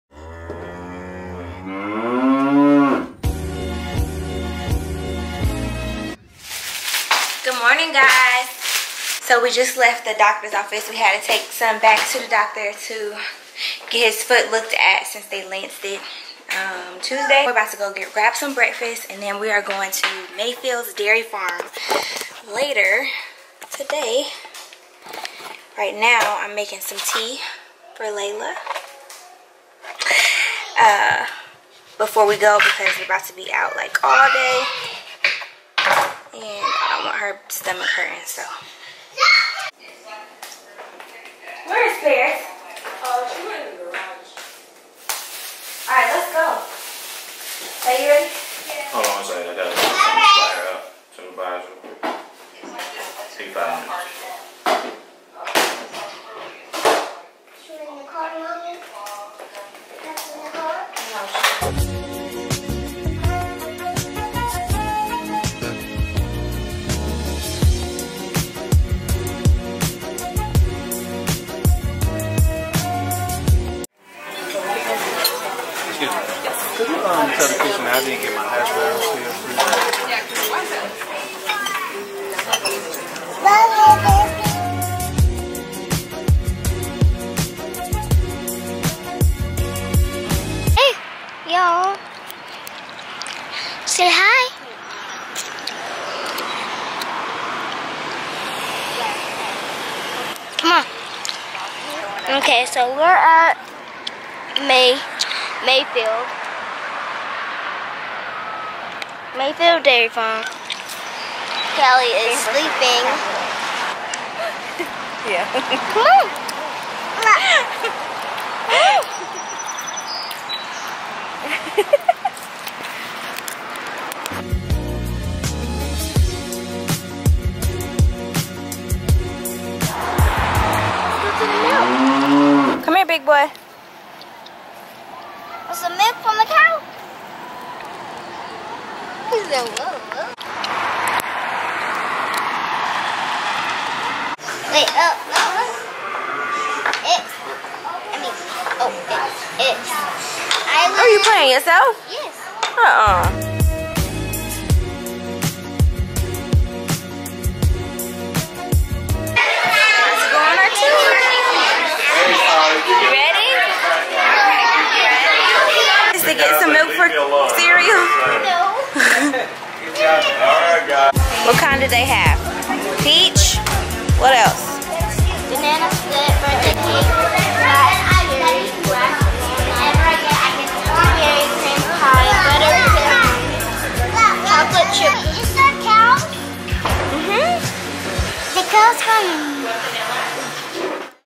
I MILKED A COW!!! | FicklinTV Daily Vlog June 26, 2017
Come with us to Mayfield Dairy Farm! We learned where milk comes from and we had a chance to milk a cow!

@MrsAFicklin (Instagram, Snap Chat, Twitter)

@KendallFicklin2 (Instagram)

@AlmightyStylez_ (IG)

Also visit our Other YouTube Channels:
MuvaFicklin
Master Key Investments